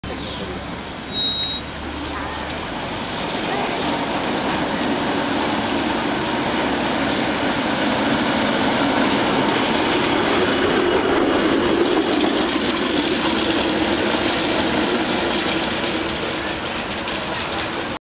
Zurich Tram Running in San Fransisco (2007)
I've seen Zurich trams both in Zurich, and in San Fransisco